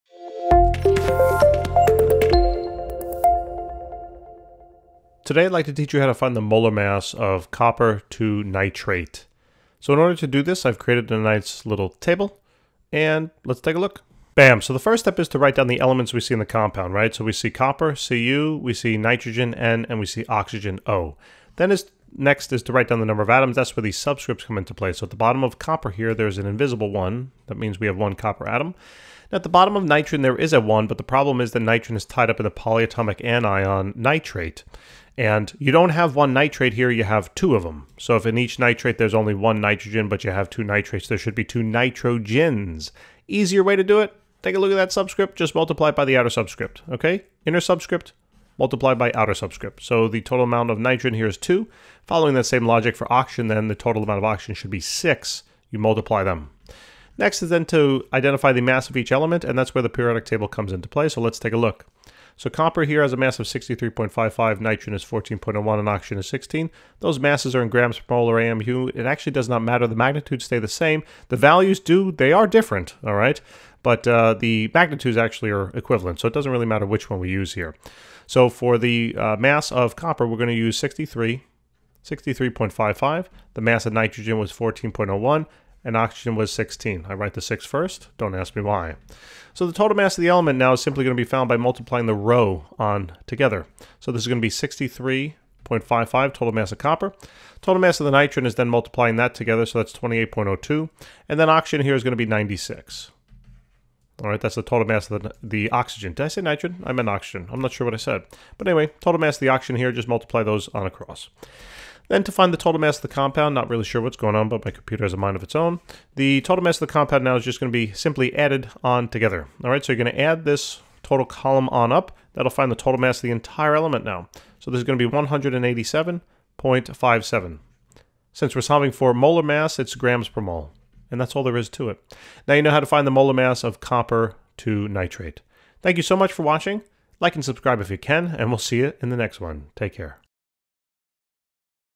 How to find the molar mass of Cu(NO3)2 (Copper (II) Nitrate)
Calculate the molar mass of the following:
Cu(NO3)2 (Copper (II) Nitrate)